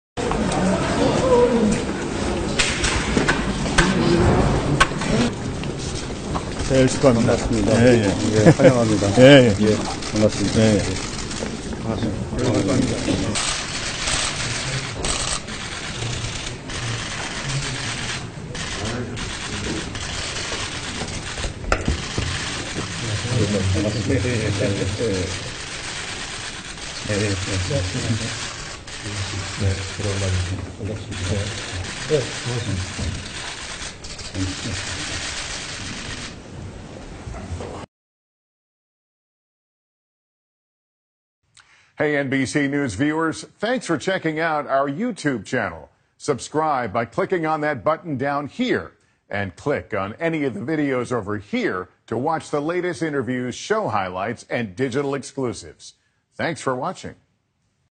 North And South Korea Meet For First Talks In 2 Years NBC News
North And South Korea Meet For First Talks In 2 Years   NBC ews